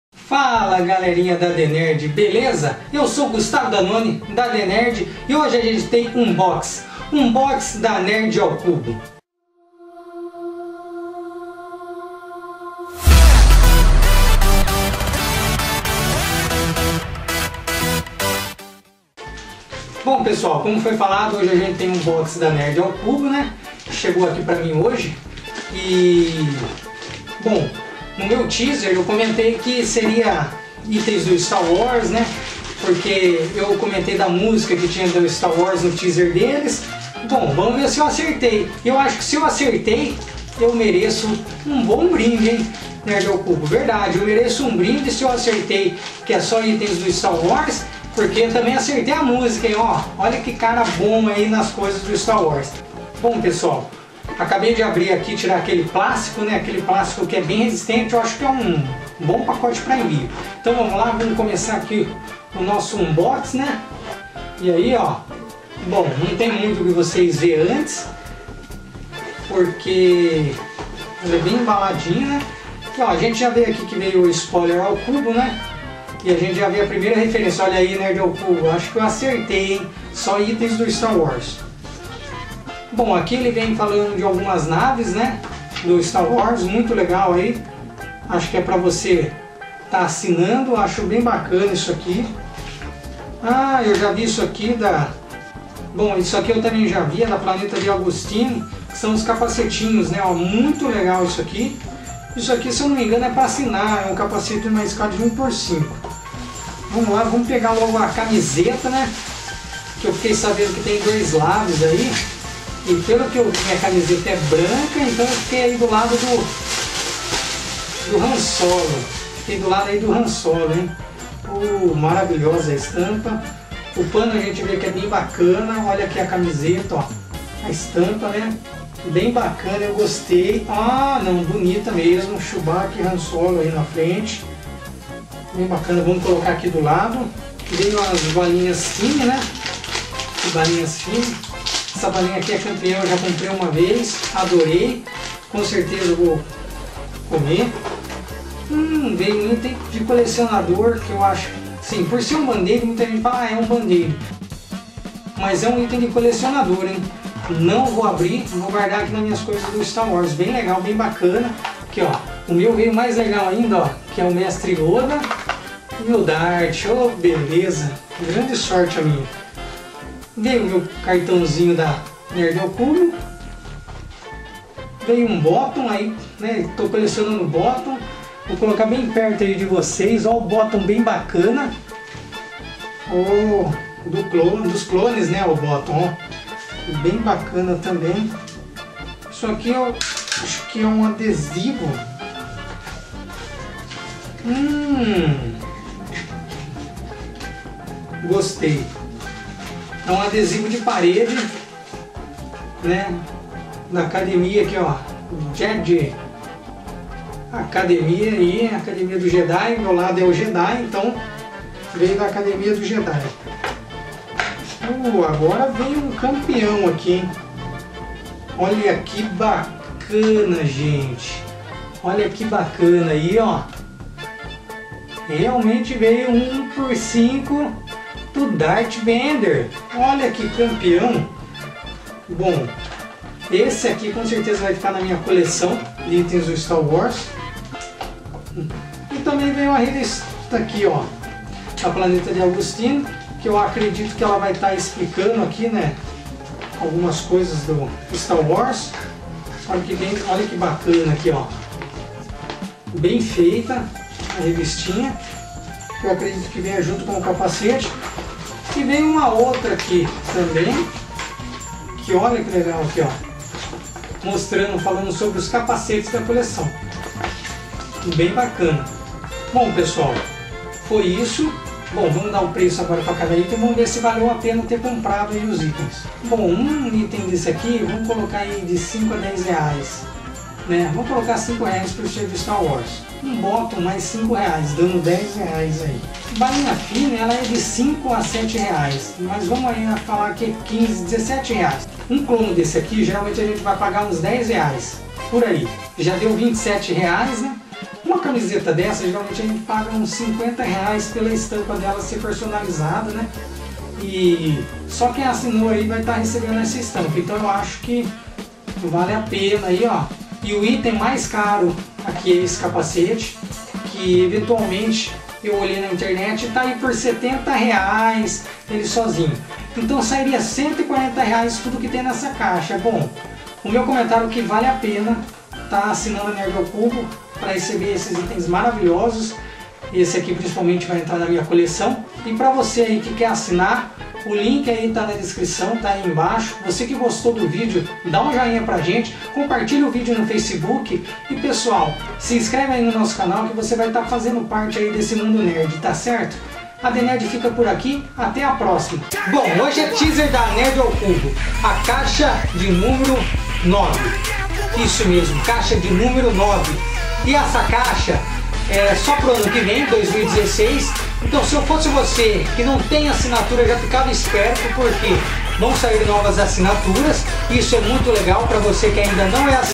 Unboxing Nerd ao Cubo #8 - GALÁXIA
Unboxing da Nerd ao Cubo #8 do tema Galáxia, confira todos os produtos que estavam dentro da caixa.

Assine Nerd ao Cubo.